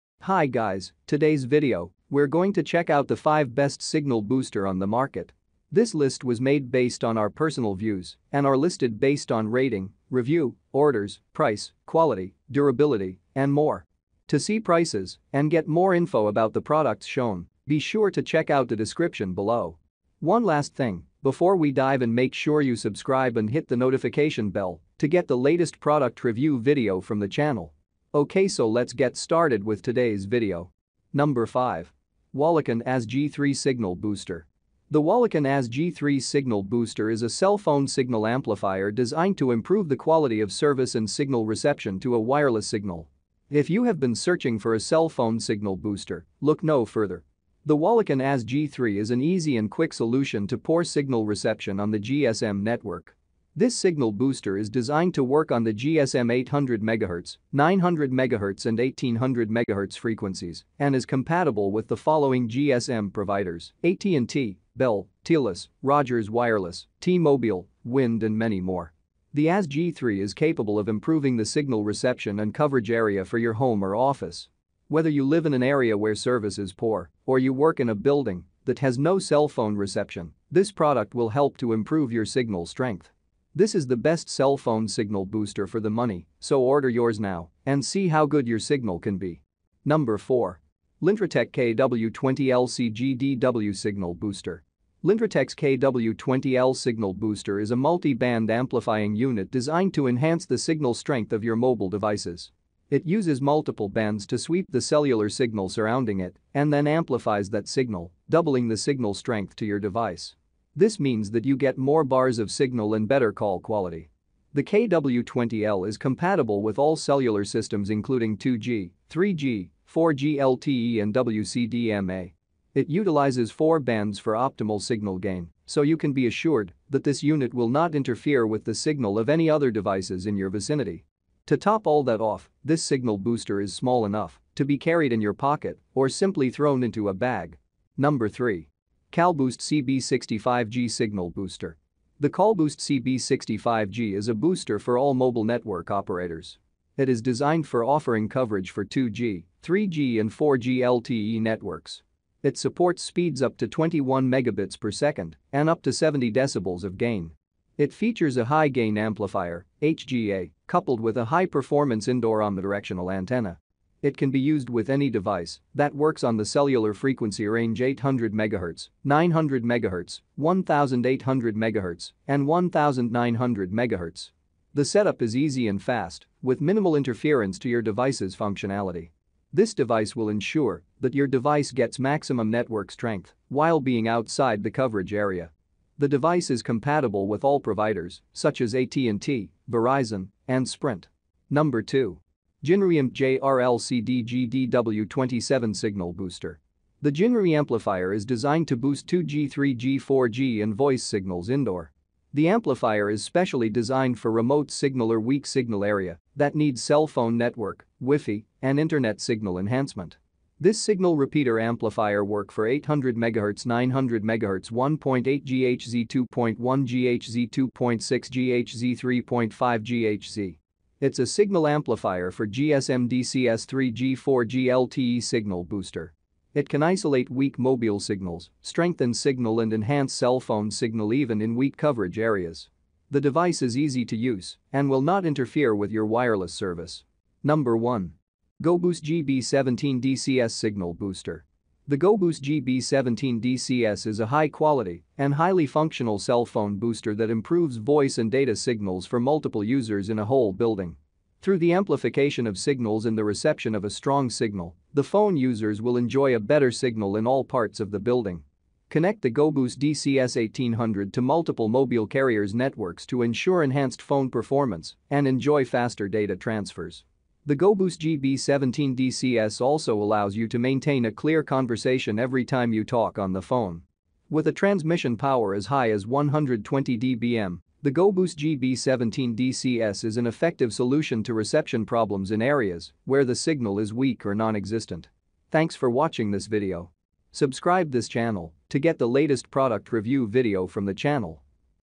Top 5 Best Signal Booster in 2022
In this video we are showing top 5 best budget cell phone booster for home and rural areas. I think you will be happy to see the review video of our Signal Booster.

5. Walokcon AS-G3 Signal Booster

4. lintratek KW20L-CGDW Signal Booster

3. Callboost CB65G Signal Booster

2. JinRuiAmp JR-LCD-GDW27 Signal Booster

1. GOBOOST GB17-DCS Signal Booster

arr reviewer